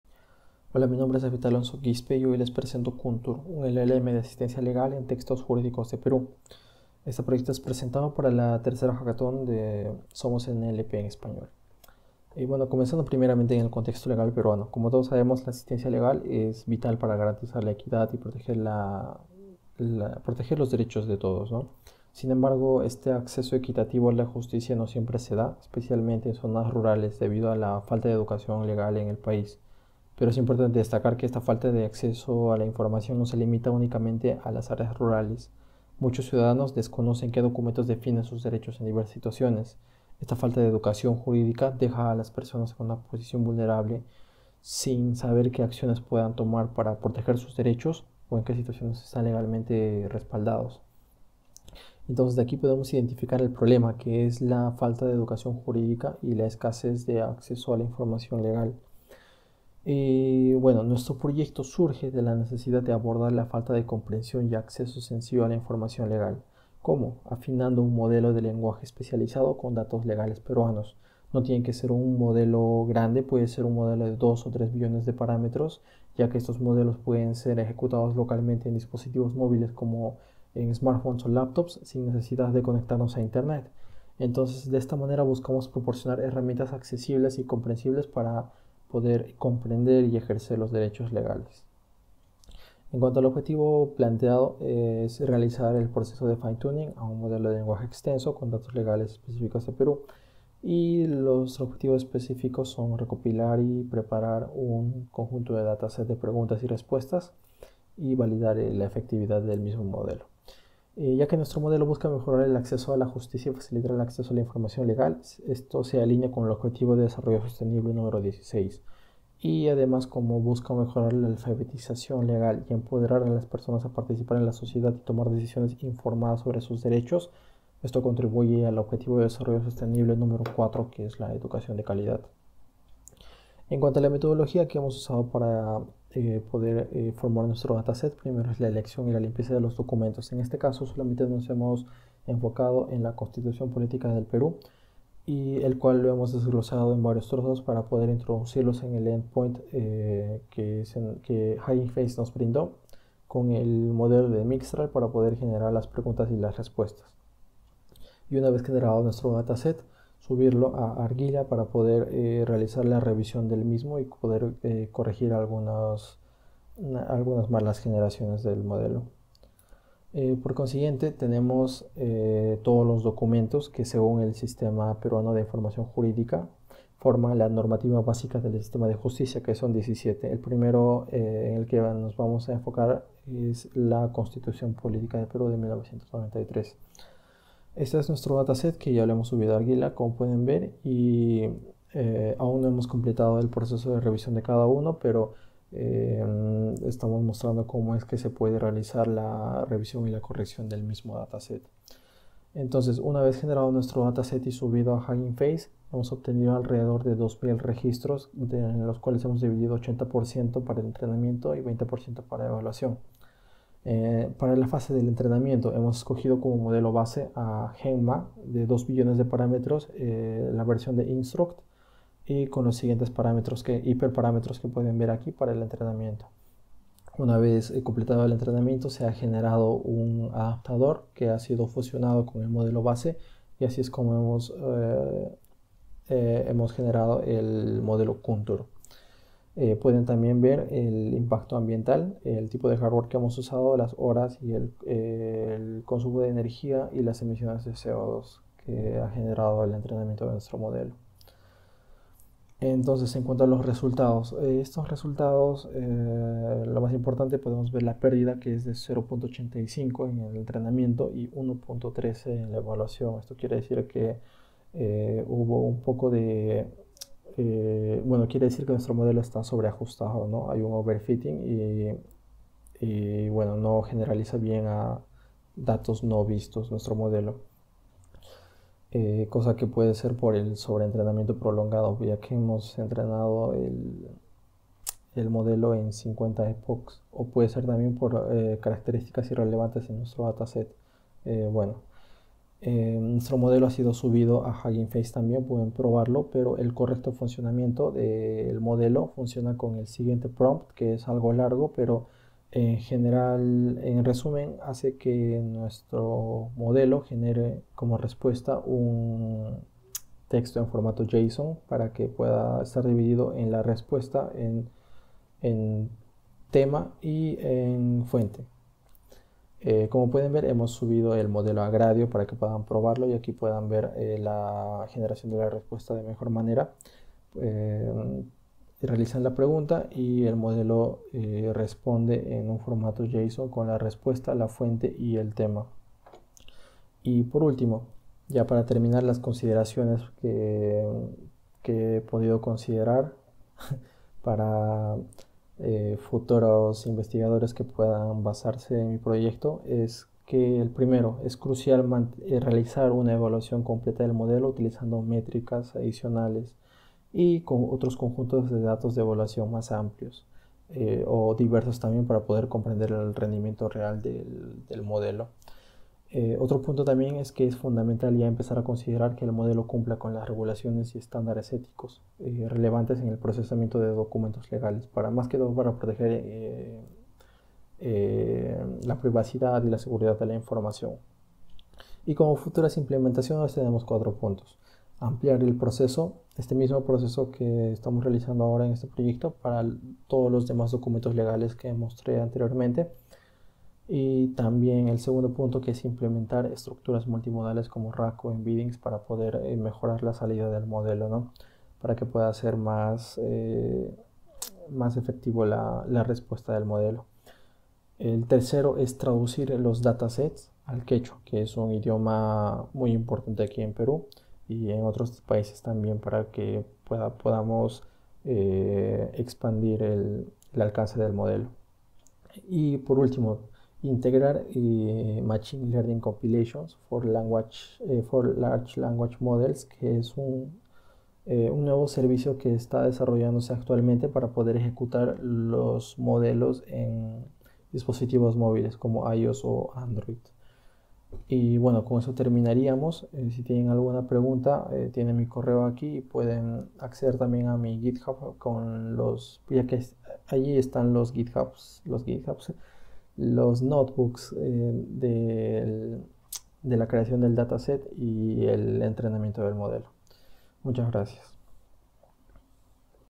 KUNTUR: Asistencia legal en Perú | Proyecto Hackathon
Nuestro proyecto busca abordar la brecha de acceso a la información legal, especialmente para aquellos en comunidades rurales y para quienes carecen de experiencia en derecho. Nos motiva la creencia de que todos deben tener la capacidad de comprender y ejercer sus derechos legales de manera efectiva. Nuestro objetivo es desarrollar un modelo de lenguaje especializado que haga que la ley sea más comprensible y accesible para todos, capacitando a individuos con conocimientos legales para tomar decisiones informadas y proteger sus derechos.